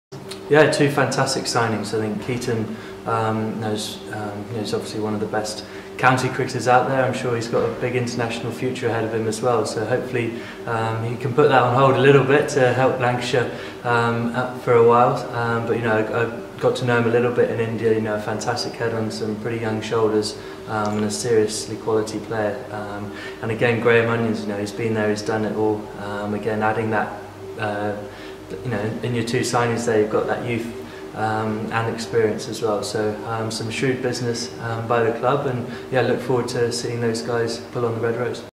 Jos Buttler on Keaton Jennings and Graham Onions
Lancashire wicket keeper Jos Buttler talks about the Club's new signings Keaton Jennings and Graham Onions.